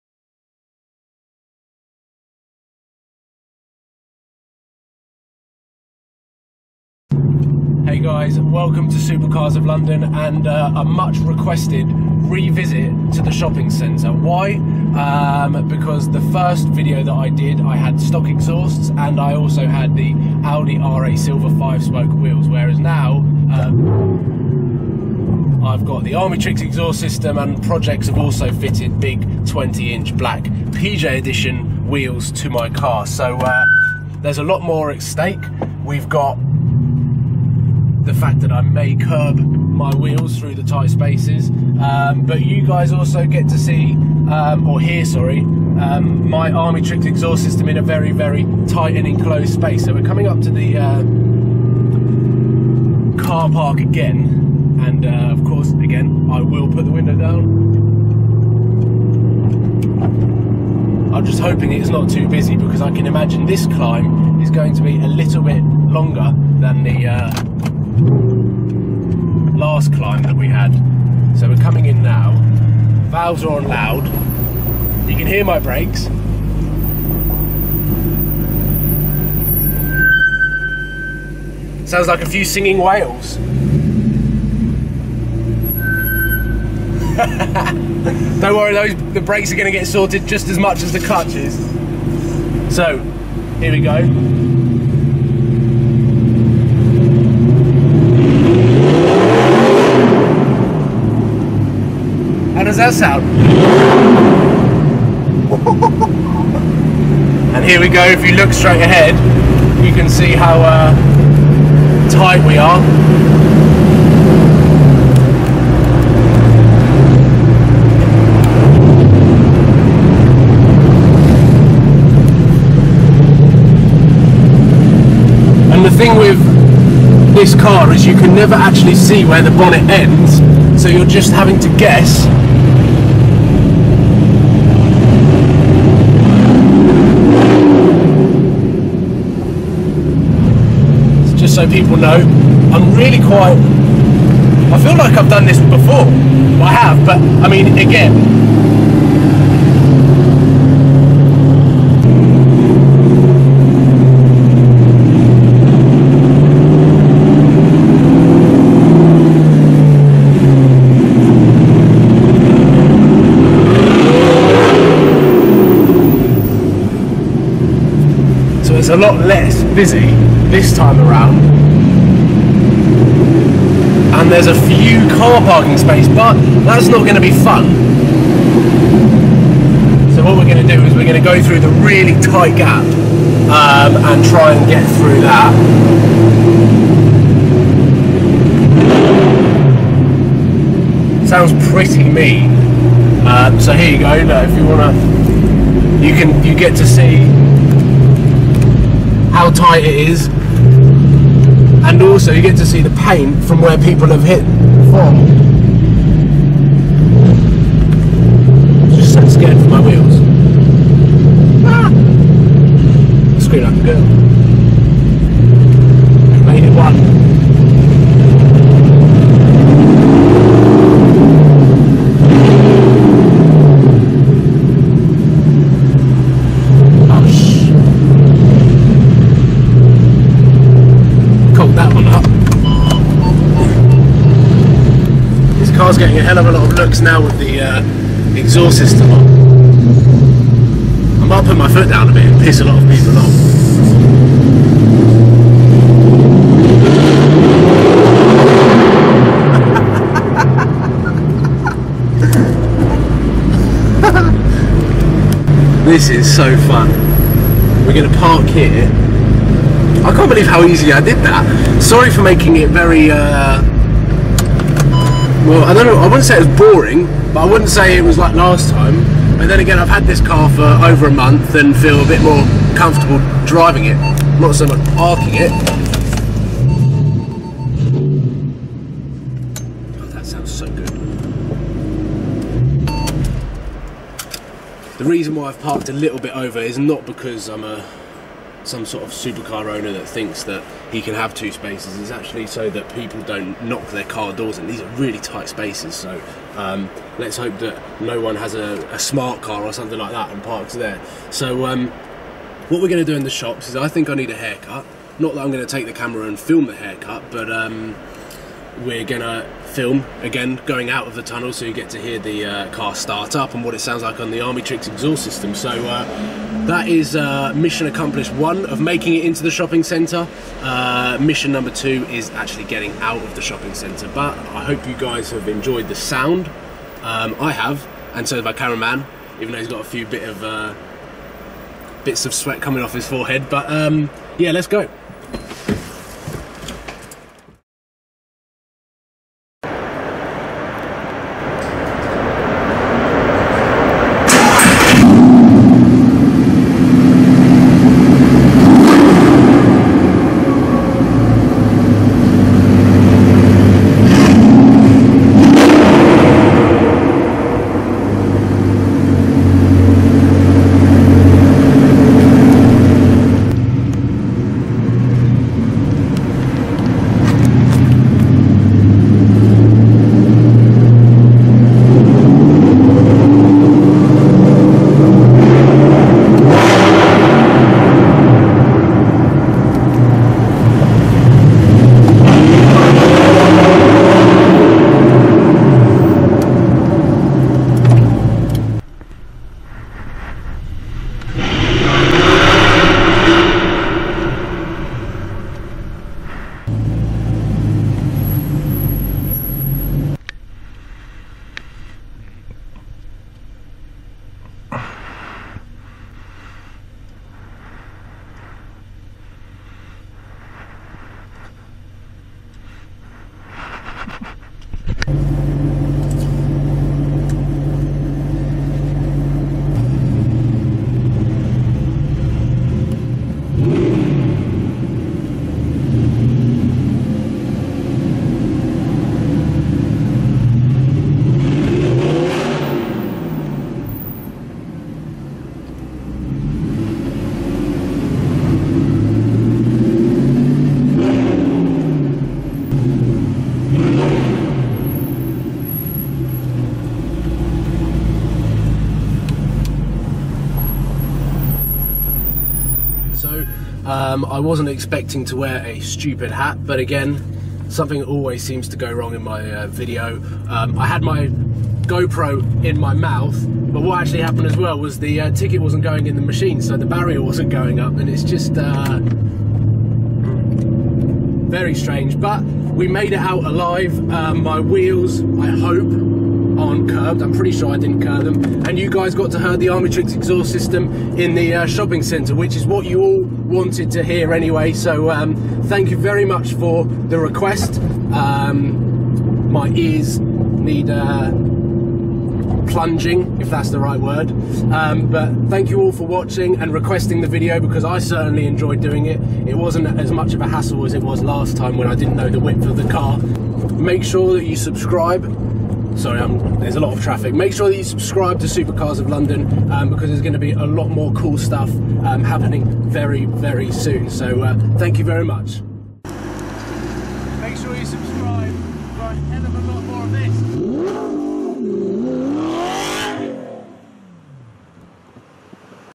Shopping with a LOUD Supercar: Armytrix Audi R8
A much requested video. Shopping with my Armytrix Audi R8! It's silly loud!!